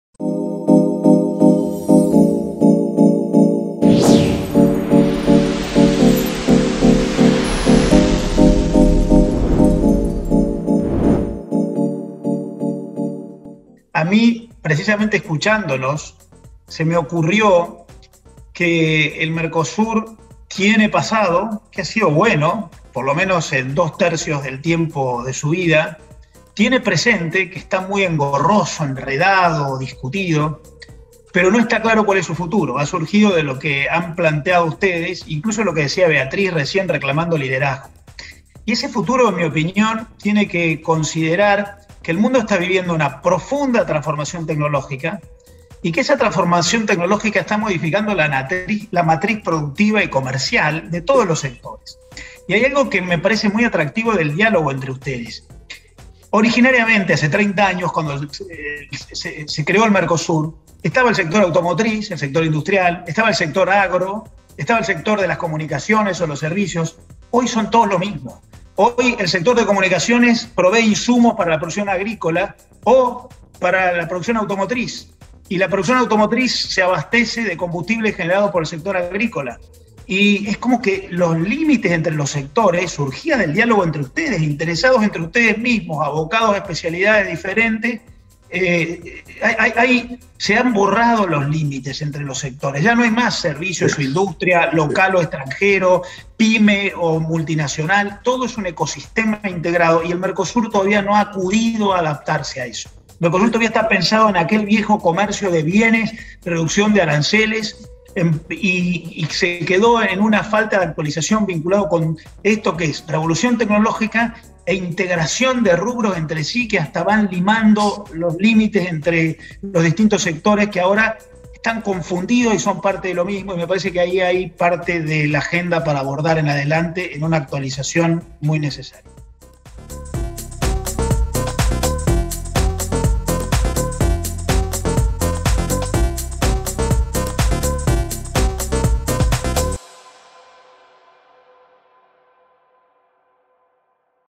LIDE Talks Internacional MERCOSUR: Presente y Futuro | Marcelo Elizondo
Abordaremos una visión de la Argentina acerca del Mercosur del Futuro con expositores de distintos sectores de producción como Agroindustria, Automotriz y Servicios